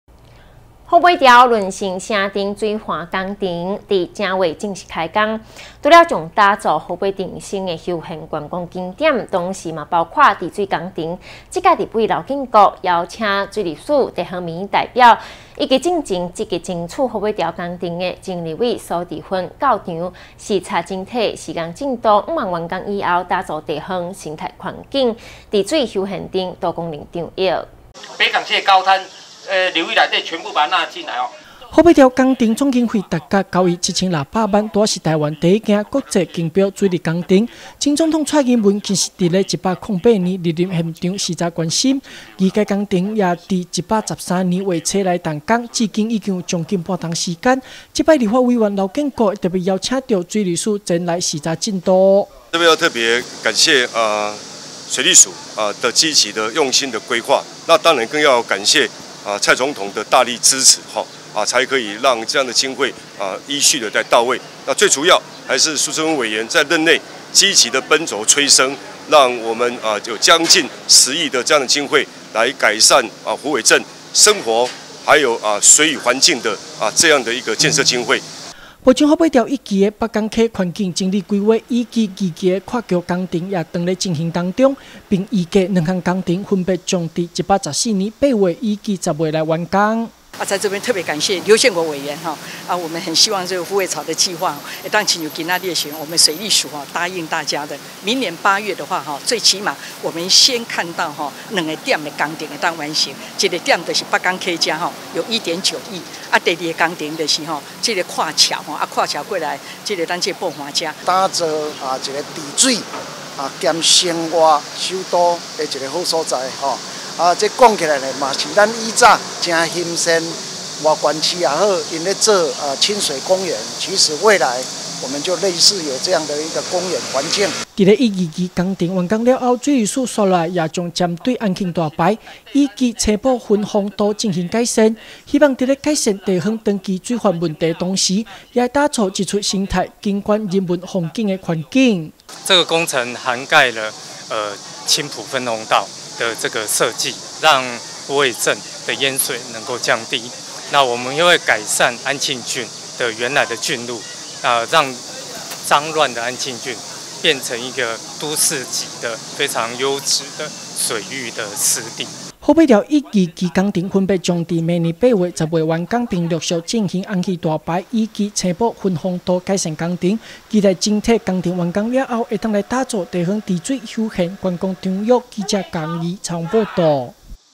劉建國邀請水利署 視察虎尾潮工程進度-雲林新聞網
虎尾潮韌性城鎮水岸工程日前在113年1月開始動工，除了將打造虎尾鎮新的觀光景點，同時也包括治水工程，成為民眾休閒遊憩的好場域。這次立委劉建國也特別邀請水利署、地方民意代表、以及先前積極爭取虎尾潮工程前立委蘇治芬來到現場視察整體施工進度，期待工程完工後，能夠打造地方生態環境、治水、休閒等多功能場域。